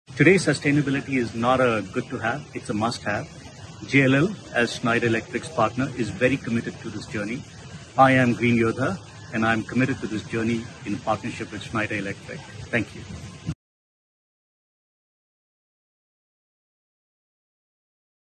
JLL Work Dynamics becomes a Green Yodha
Today sustainability is not a good to have, it is a must-have for any organization.”
JLL Work Dynamics join us in the Good Fight to become a GreenYodha!

Get started on your organisation's sustainability journey. Know More